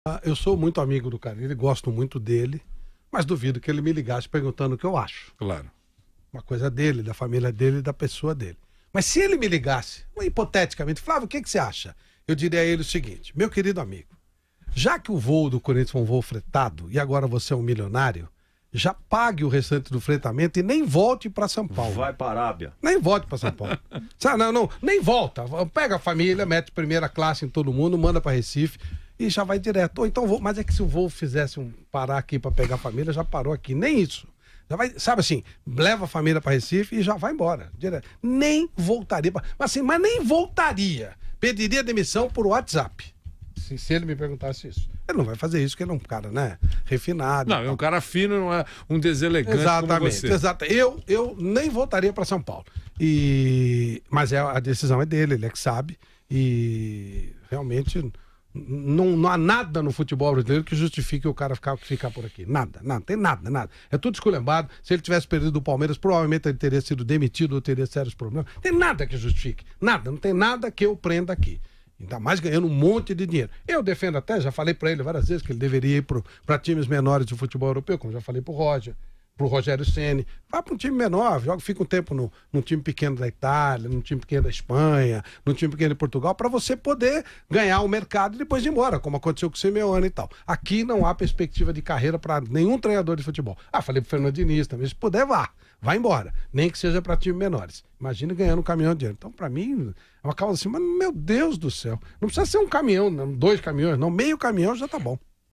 Sair ou ficar? OLHA o que Flavio Prado diria a CARILLE!
Fábio Carille pode deixar o Corinthians e fechar com o Al-Hilal. O comentarista Flavio Prado, da Rádio Jovem Pan, analisou a possível troca.